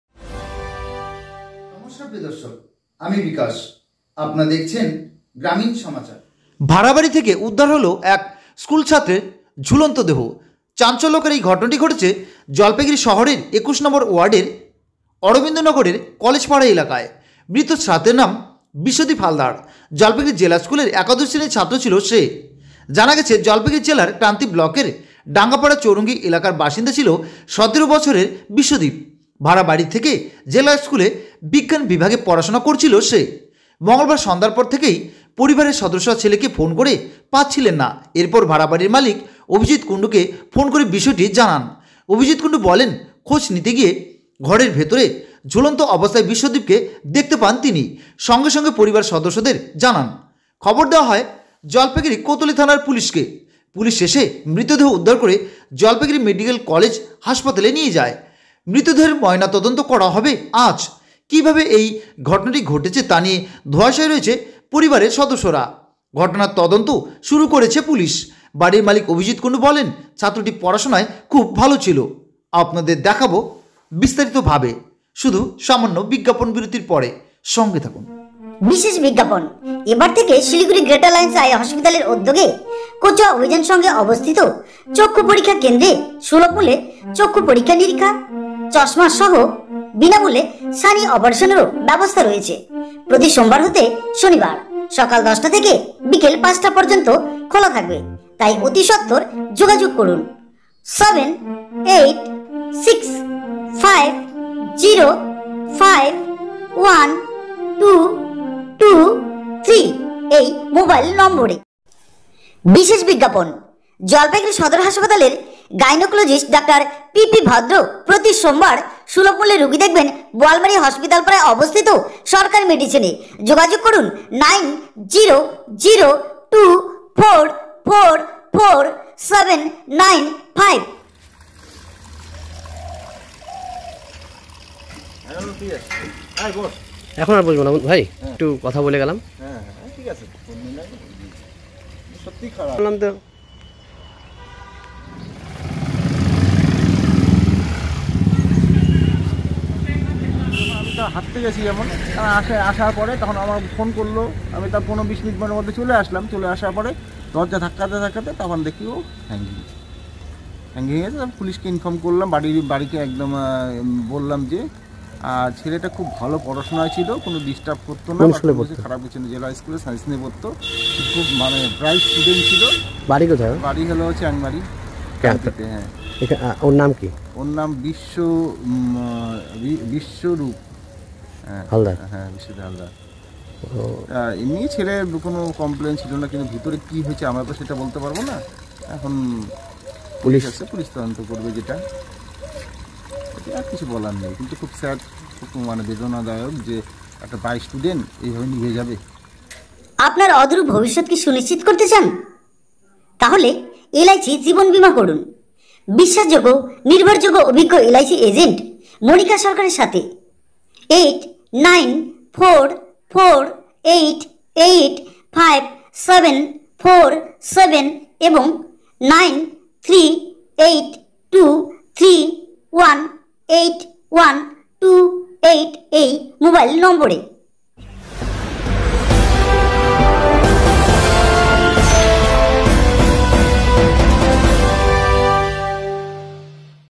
জলপাইগুড়ি ভাড়া বাড়ি থেকে উদ্ধার হলো এক স্কুল ছাত্রের ঝু*লন্ত দেহ , দেখুন বিস্তারিত।
জলপাইগুড়িতে ভাড়াবাড়ি থেকে উদ্ধার হলো এক স্কুল ছাত্রের ঝু*লন্ত দেহ, ঘটনায় ব্যাপক চাঞ্চল্য।